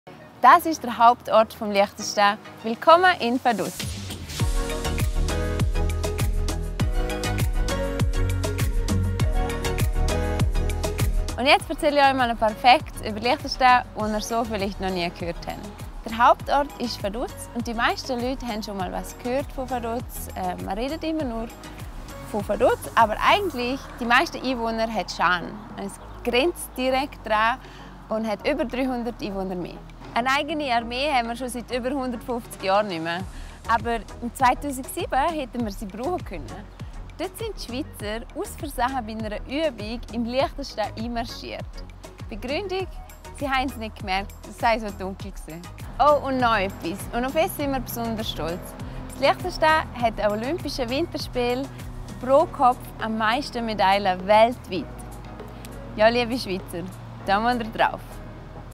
Tinas Lieblingsplätze entlang des Liechtenstein-Wegs: Fussgängerzone in Vaduz
Die liechtensteinische Ex-Skirennfahrerin Tina Weirather zeigt euch diesen Sommer ihre Lieblingsplätze entlang des Liechtenstein-Wegs. Der Weg führt auf 75 km durch das ganze Fürstentum und wird idealerweise in 2, 3 oder 4 Etappen absolviert. Die dritte Station führt Tina in die Fussgängerzone vom Hauptort Vaduz. Dort befinden sich auf einer Länge von wenigen hundert Metern beeindruckende Architektur, historisch und modern, Kunst, Museen, Shopping-Möglichkeiten, Restaurants und viele Sehenswürdigkeiten. Im "Städtle" zählt Tina einige Fakten über Liechtenstein auf, die bestimmt nicht jeder weiss.